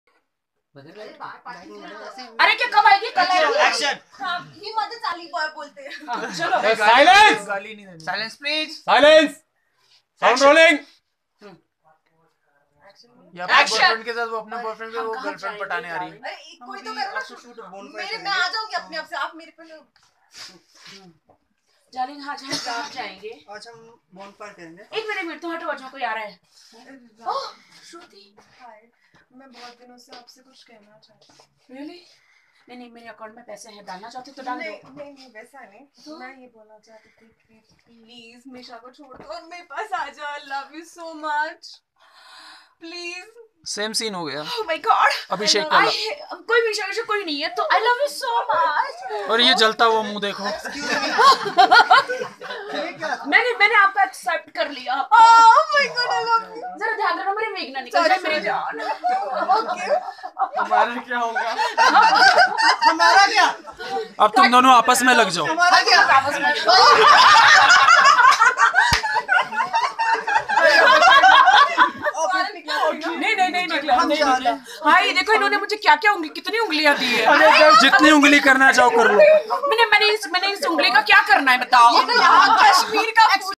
Rakhi Sawant | Pending Love | Bollywood | Actor Mohit Sharma | Off shoot Masti | Shrutika Gaokkar
During the shooting of Bollywood Movie "Pending Love "
-Mohit sharma